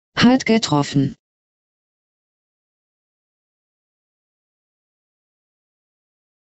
hat getroffen
This is the Microsoft Hedda voice that is reading the word hat getroffen